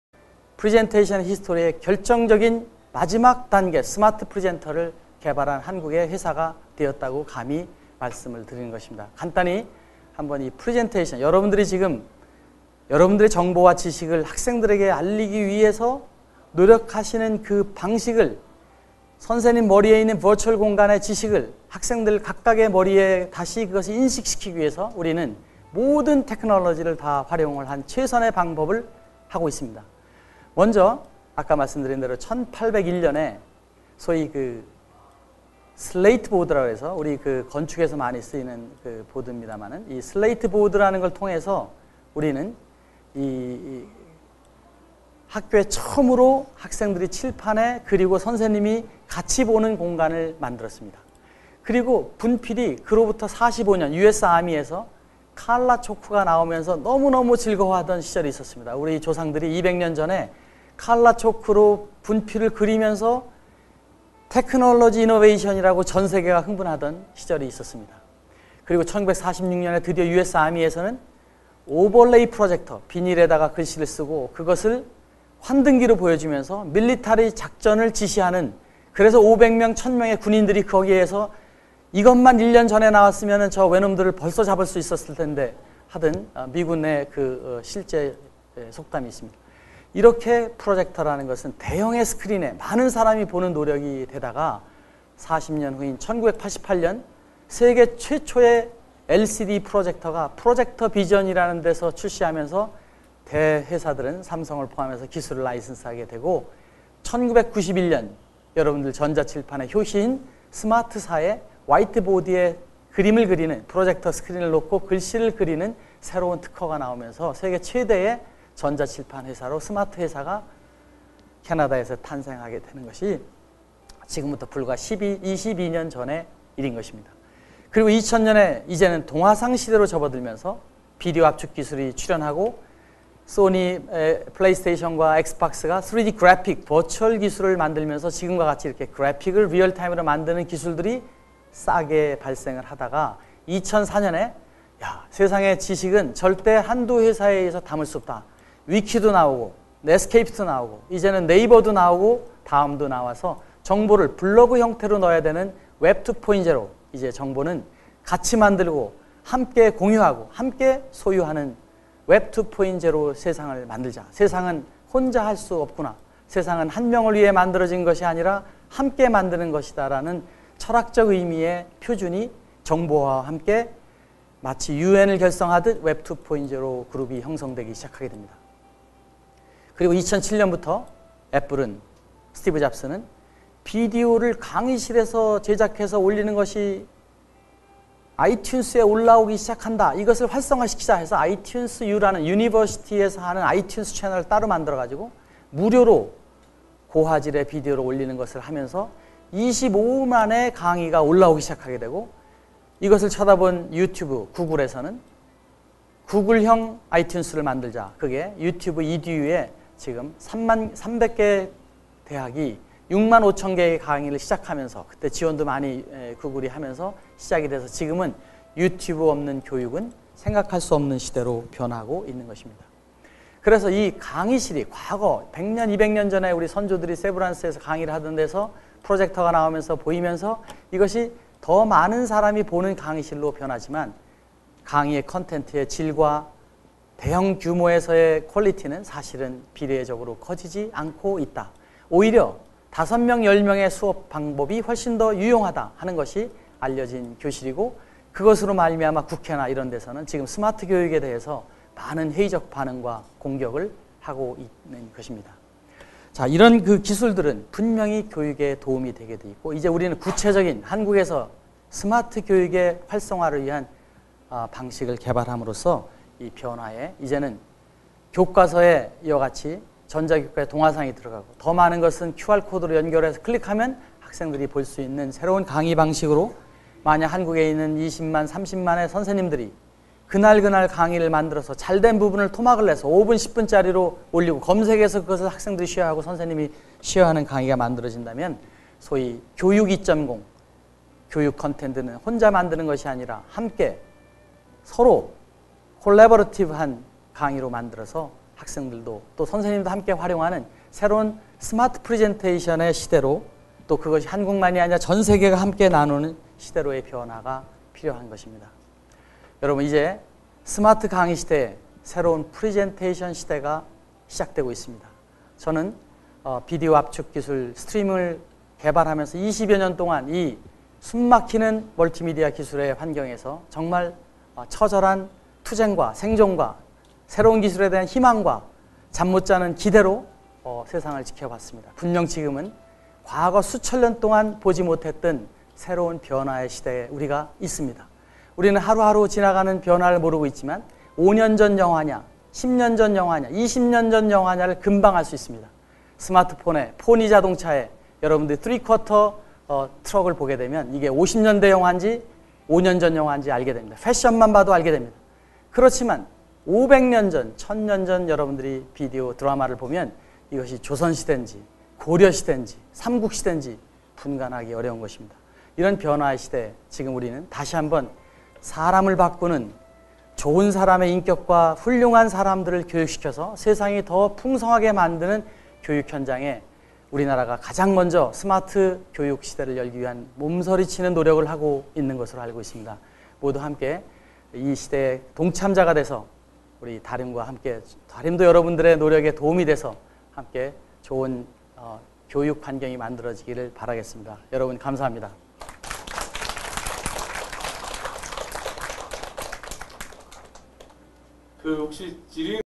프리젠테이션 역사와 iStudio에 의한 참여-공유-개방의 교육 2.0 ( 연수 마지막부분 정리)
교실 수업으로 학교  교육의 칠판 수업이 200년 전에 시작되어 50명, 100명의 교실수업이 이루어지고 프로젝터의 대중화로 300, 1000명이 되는 시대로, 다시 인터넷과 스마트 장치로 수천 수만명이 가능한 시대로의 변화가 이루어 졌다.  이러한 스마트 혁명의 교육의 현장에서 교실을 Open하고 강의를 영원히 사이버 상에 공유하는 교육 2.0 시대를 가능하게 하고 있다.